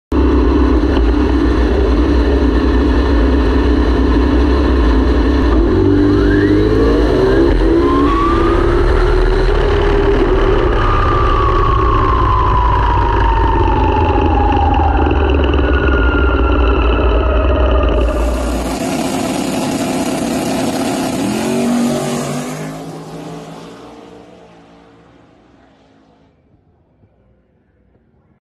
1.01 60” on a DRAG RADIAL!
275/60/15 Mickey Thompson Tires doing what they do best!

Frank Mewshaw’s Pro-275 Firebird making a 3.86 pass at 197 MPH 🤘

1.01 60’